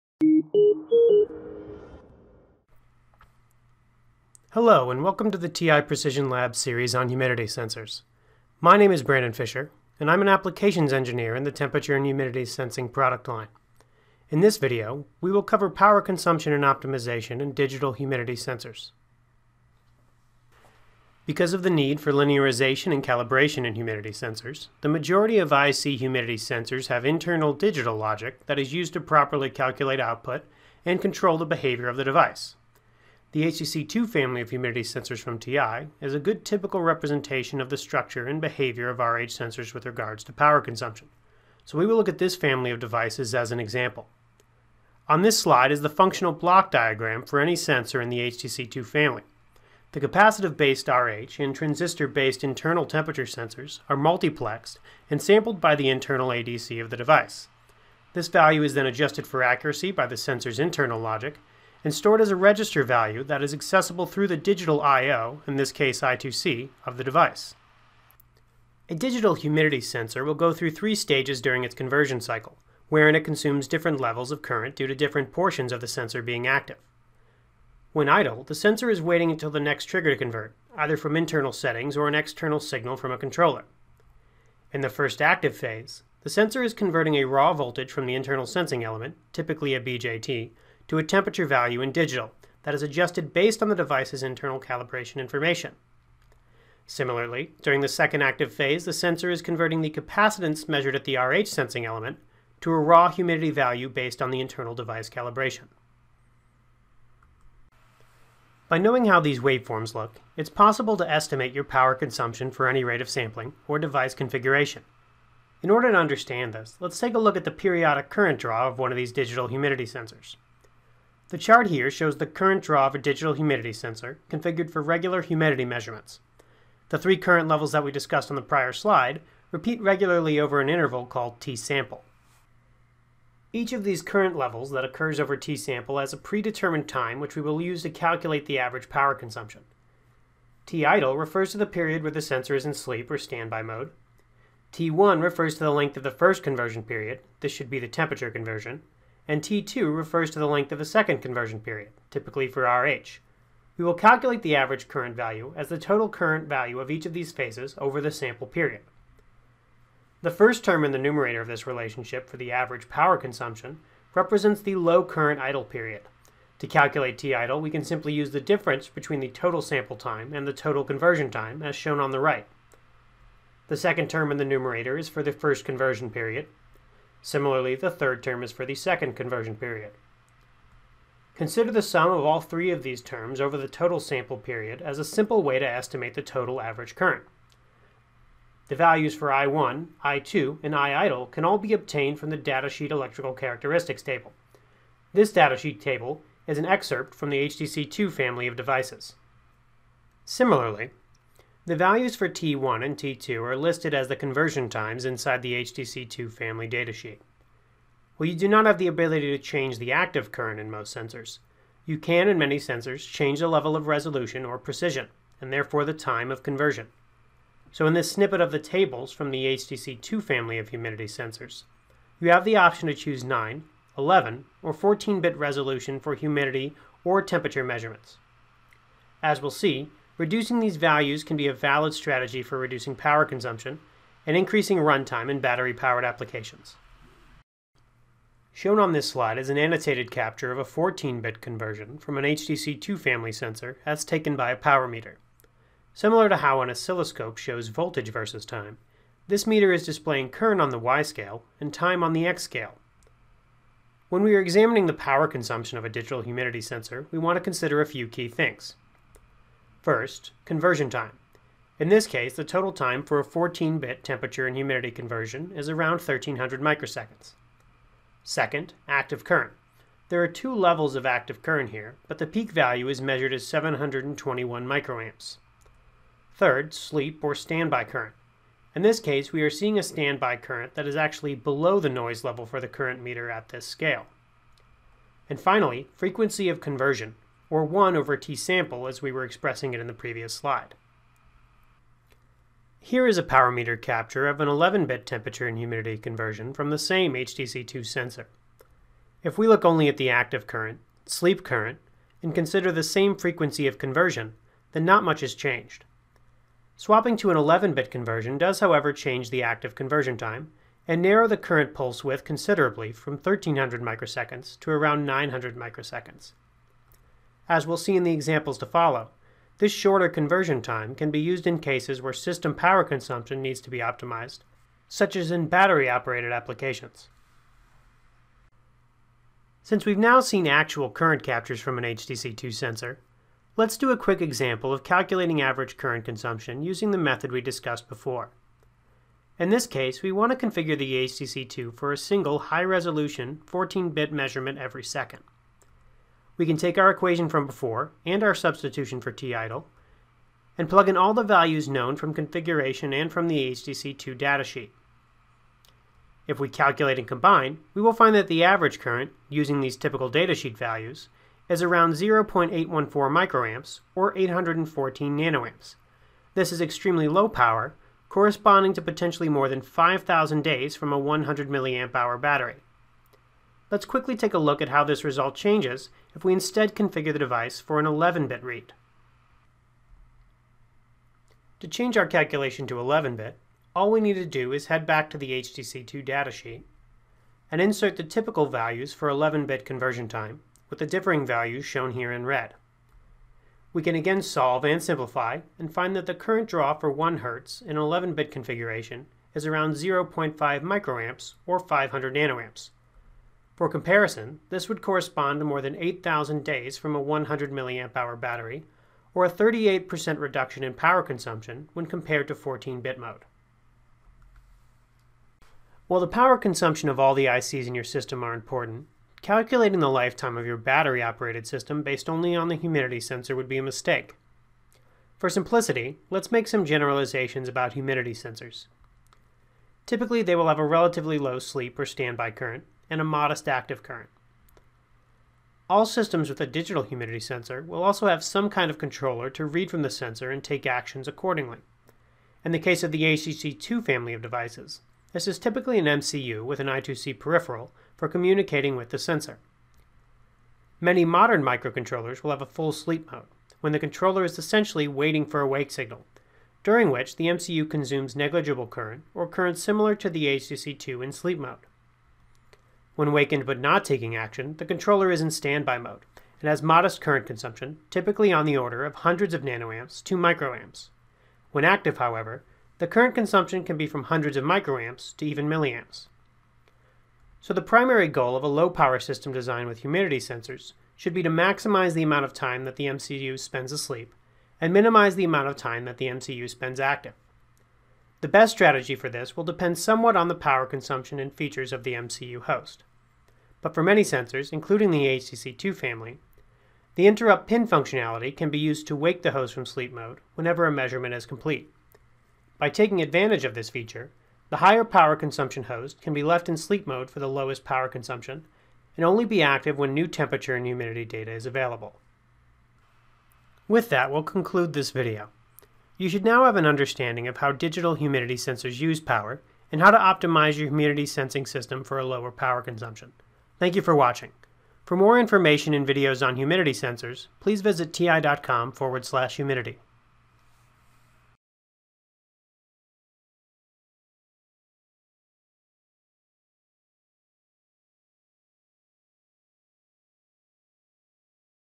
Digital humidity sensor power consumption
View humidity sensor ICs

This section of the TI Precision Labs - Humidity sensors series explains how
digital humidity sensors consume power, how to calculate average current and
techniques for optimizing for lower power.

Ask questions and interact with the authors on our E2E™ community